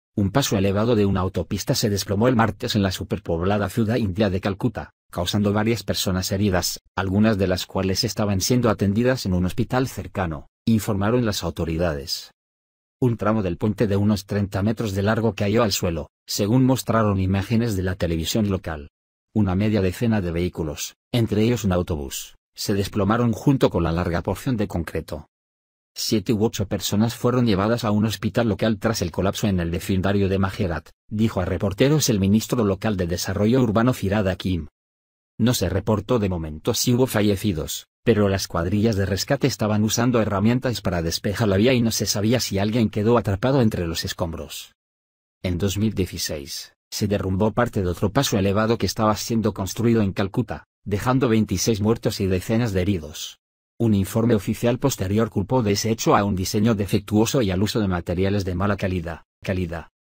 Se desploma puente en autopista de Calcuta India
Un paso elevado de una autopista se desplomó el martes en la superpoblada ciudad india de Calcuta, causando varias personas heridas, algunas de las cuales estaban siendo atendidas en un hospital cercano, informaron las autoridades.